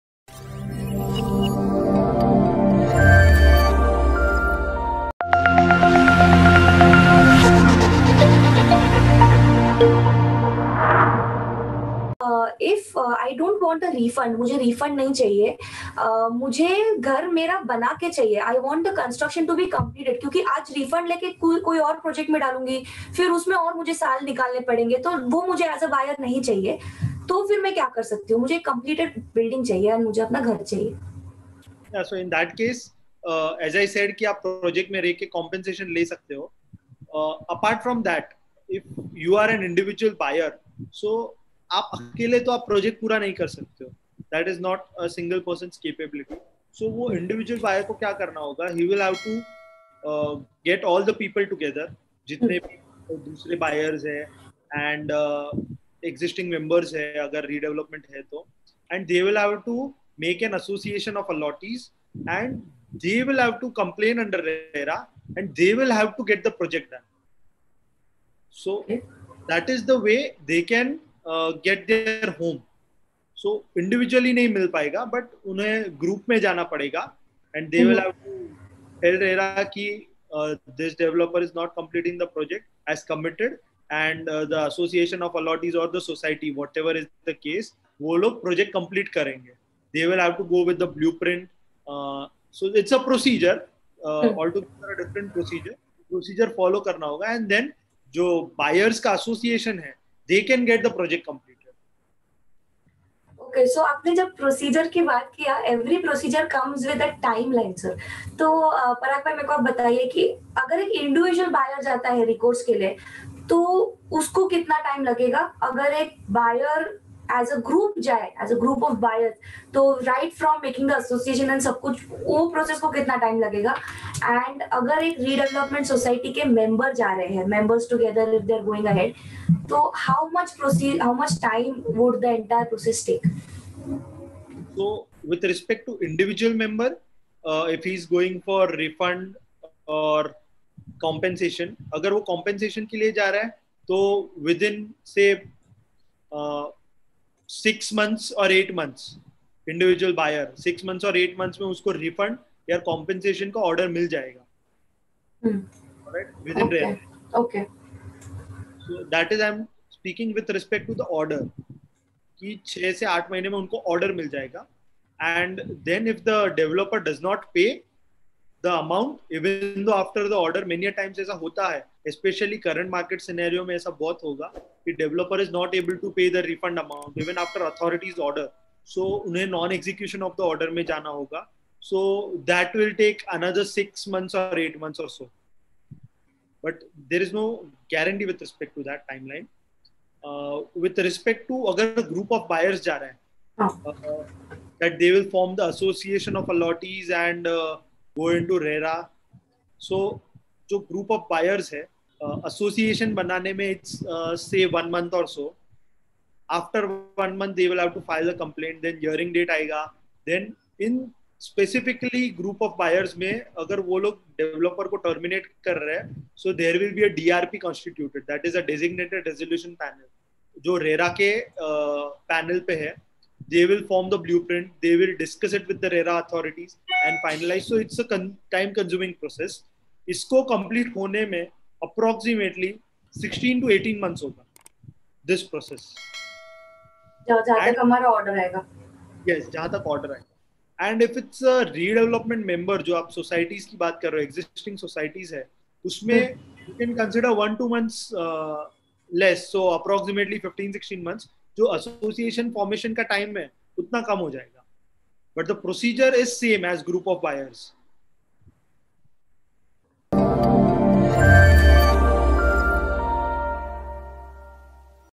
Under RERA Can a buyer demand completion of project instead of refund?
Mr. Parag Shah, CA, RERA Consultant. On Talk Show with Toughcons Swanirman. Clarifies about the stalled project where the buyer doesn't want refund & wants the completion of the project.